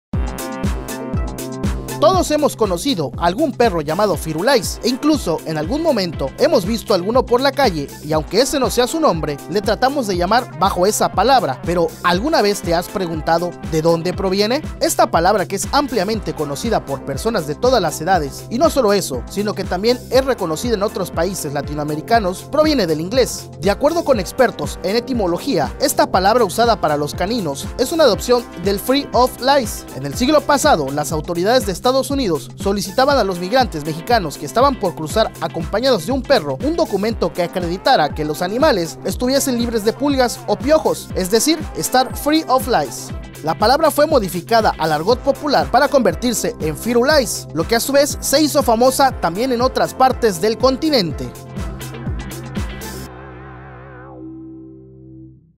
Por esta razón se nombra a los perros Firulais
Todos hemos escuchado que existen perros a los que les dicen "Firulais" e incluso en algún momento hemos visto alguno por la calle y aunque ese no sea su nombre tratamos de llamarlo con esta palabra, pero ¿alguna vez te has preguntado de dónde proviene?.